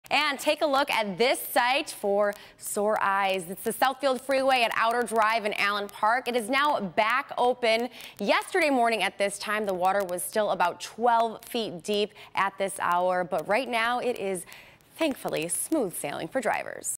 Look: Southfield Freeway back open after severe flooding
The freeway was under 12 feet of water.